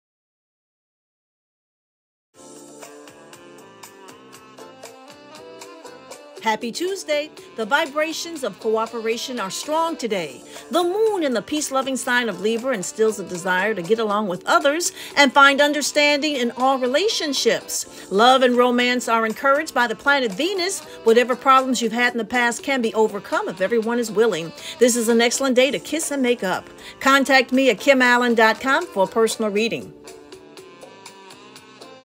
Daily Cosmic Energy for 8/2/22/ Daily Astrology by Kim
Astro Reading for Tuesday, August 2, 2022

Happy Tuesday! The vibrations of cooperation are strong today. The Moon in the ‘peace-loving’ sign of Libra instills a desire to get along with others and find understanding in all relationships. Love and romance are encouraged by the planet Venus. Whatever problems you’ve had in the past can be overcome if everyone is willing.  This is an excellent day to kiss and make up. Major adjustments can be made as ‘changeable’ planet Uranus joins Venus. Your evening activities are full of passion as the planet Mars also joins Venus in the late night hours. Surely, there will be sparks flying in romance. Best signs for the day are Libra and Aquarius. And if today is your birthday, happy birthday! You are a Leo who is surrounded by love.  Monday is your best day.